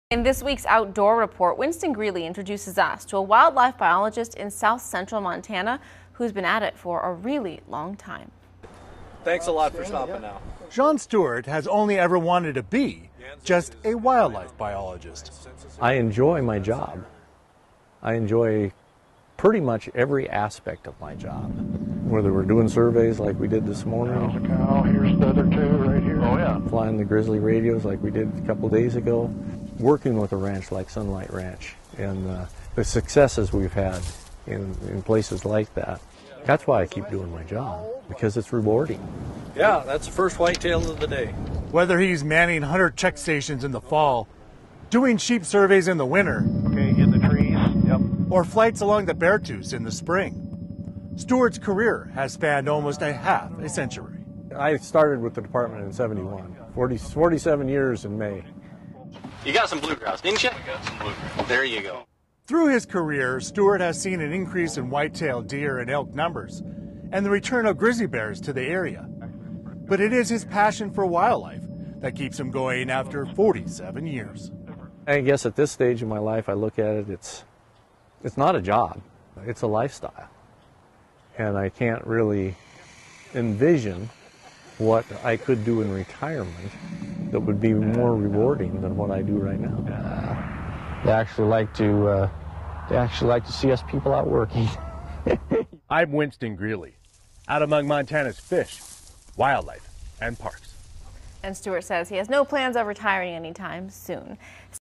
Long time Montana wildlife biologist has no plans to retire
Shawn Stewart has only ever wanted to be just a wildlife biologist.